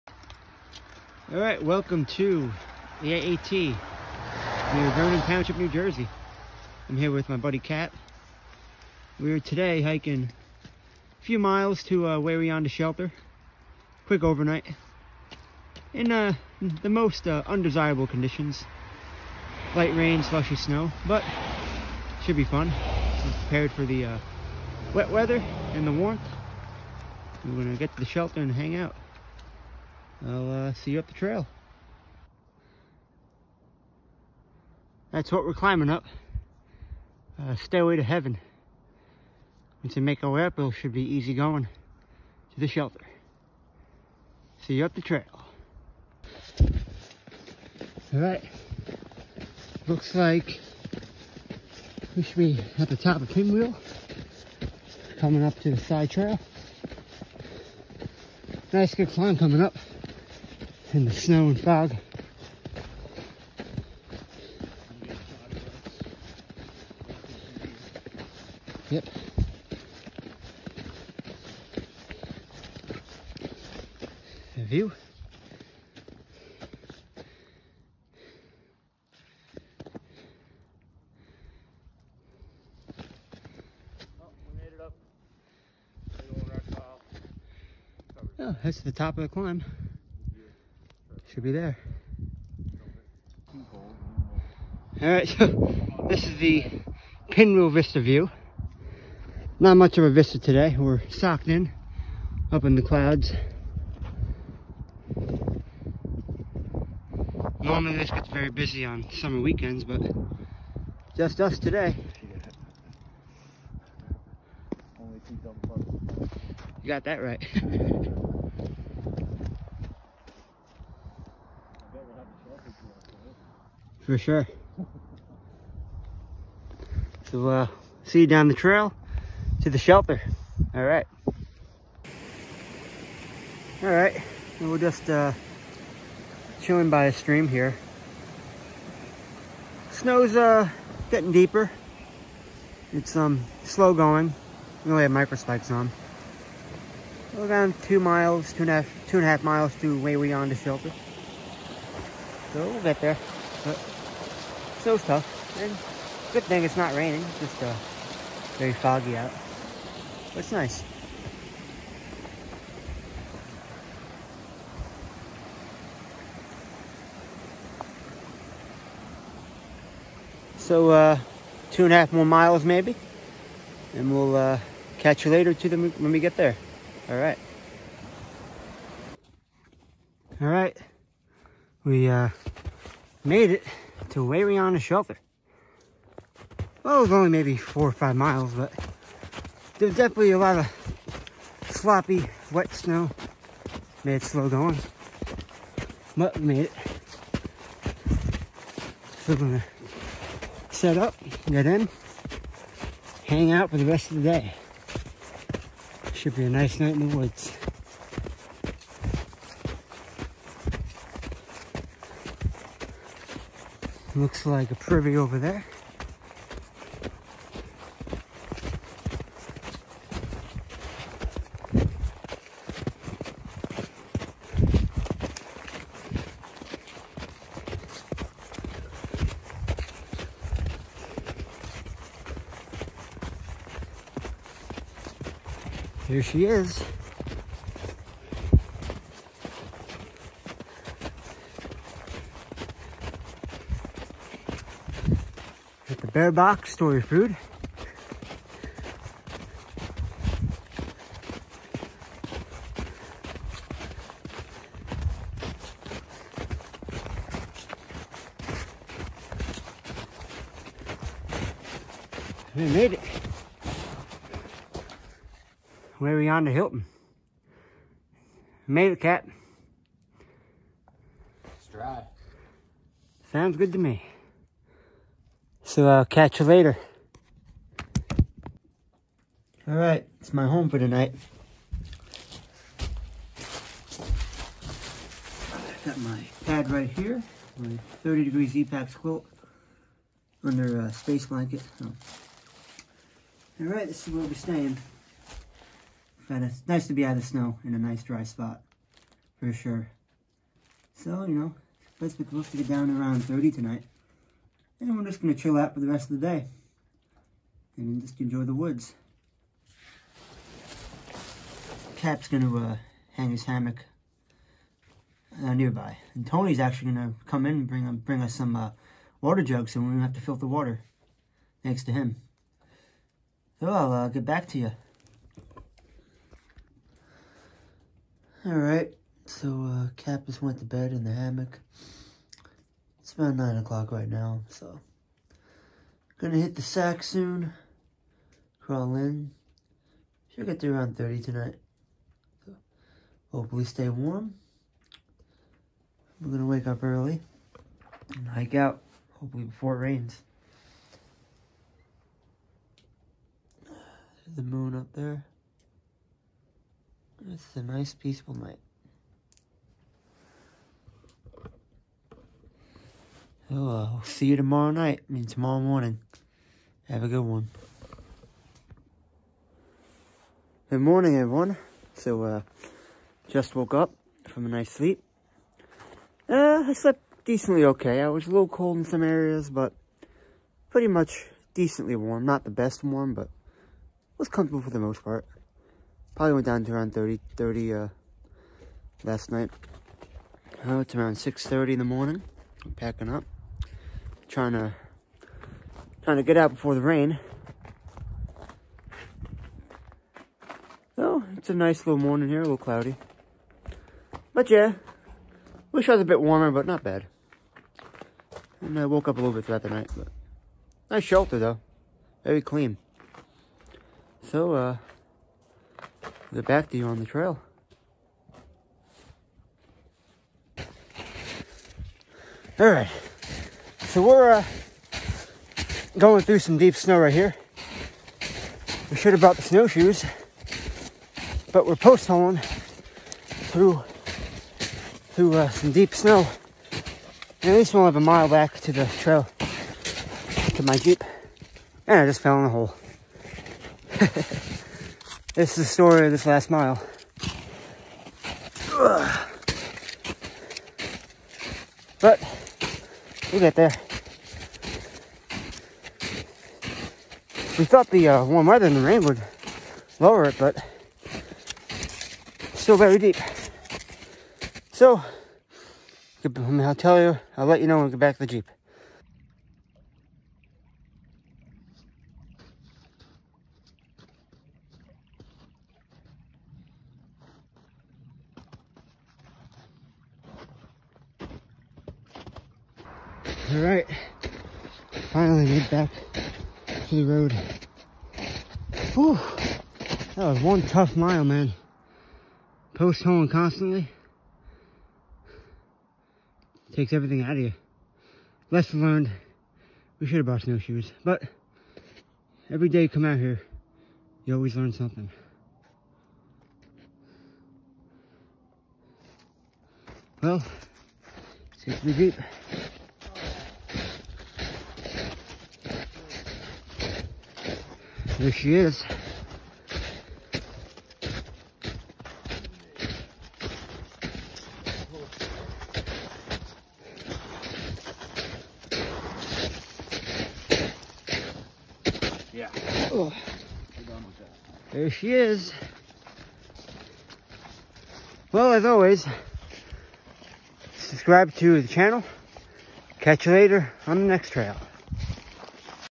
Appalachian trail overnight to Waywayanda Shelter. Cold wet and snow.
Backpacked a bit of the AT in NJ with my friend Kap. Wet cold and deep snow.  but all in all good fun